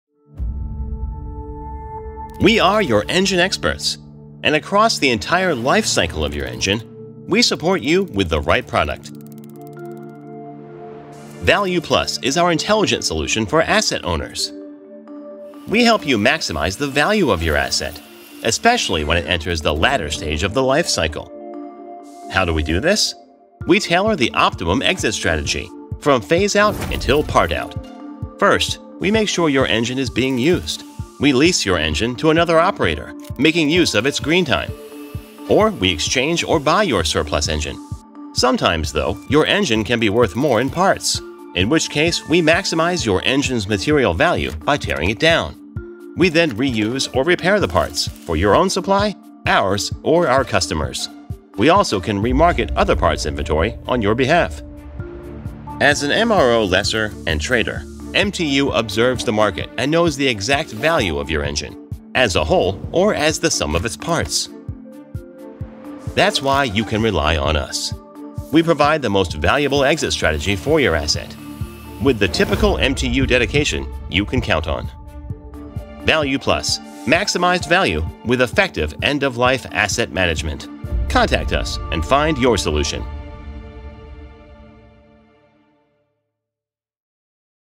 MTU Maintenance intelligent solutions: VALUEPlus
VALUEPlus is a solution for asset owners to maximize value with effective asset management at end-of-life. Exit strategies can range from generating additional income through green-time lease out to sale, exchange and trade all the way to maximizing the material value of the asset through teardown and material management.